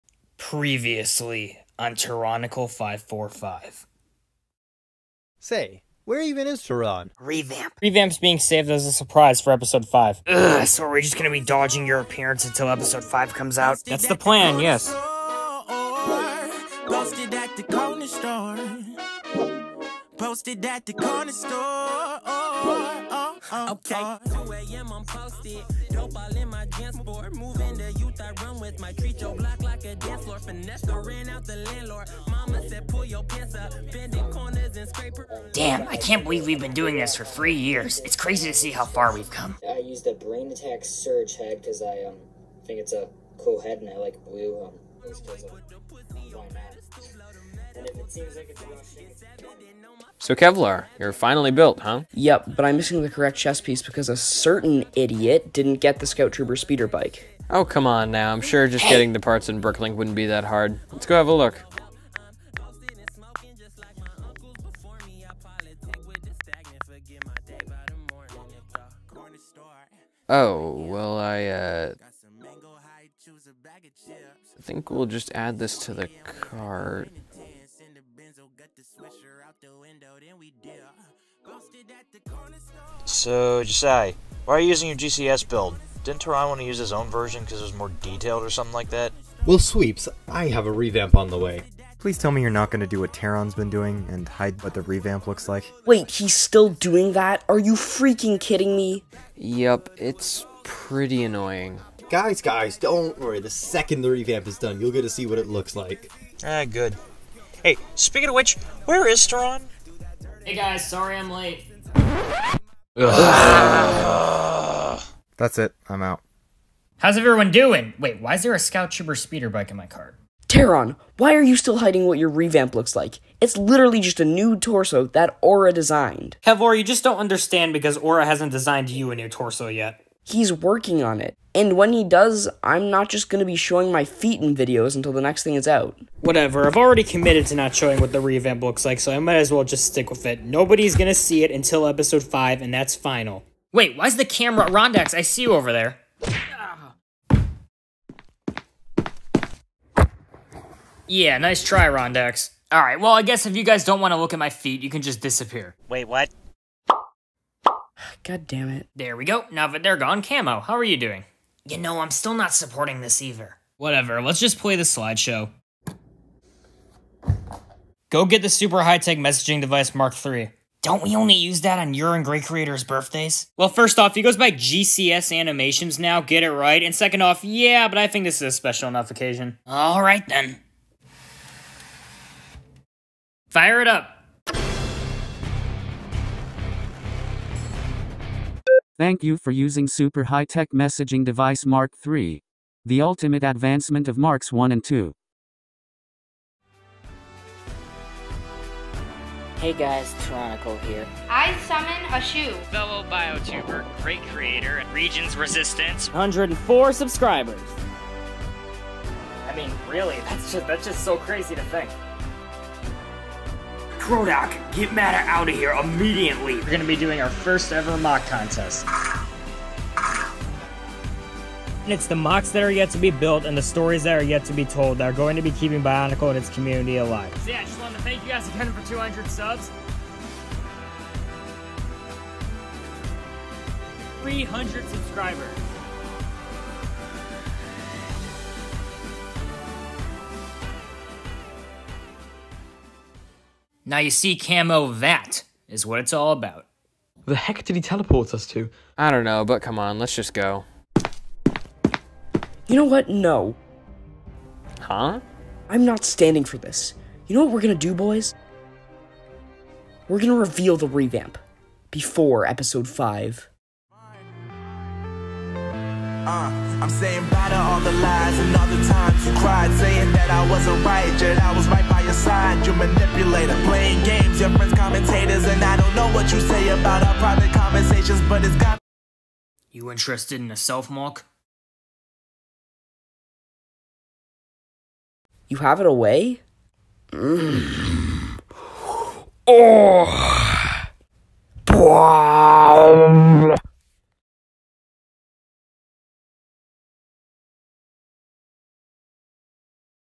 3 Years
It’s the Terronicle 545 three year anniversary party!